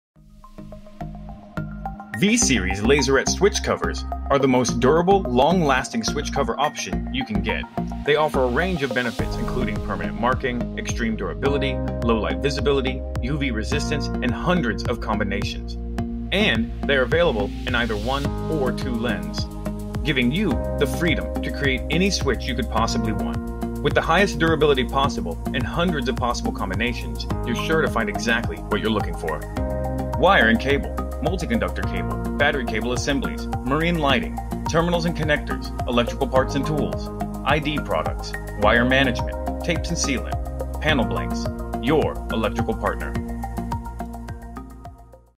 Laser Etched Switch Covers
Laser Etched Switch Covers offer a range of benefits you won't find with other types of switch covers.